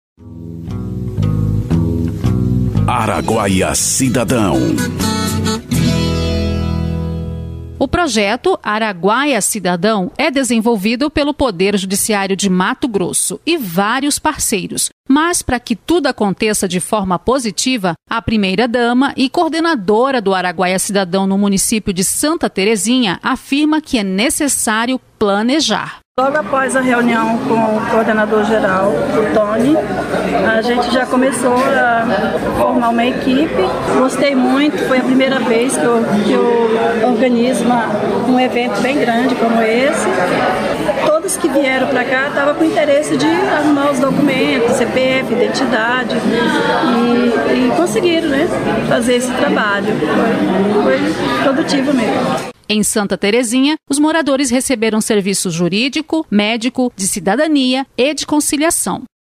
Estação TJ! Araguaia Cidadão: Primeira dama de Santa Terezinha elogia projeto
O projeto Araguaia Cidadão é desenvolvido pelo Poder Judiciário de Mato Grosso com a ajuda de vários parceiros. E para que tudo aconteça de forma positiva, a primeira dama e coordenadora do Araguaia Cidadão, do município de Santa Terezinha, afirma que é necessário planejar. Ouça!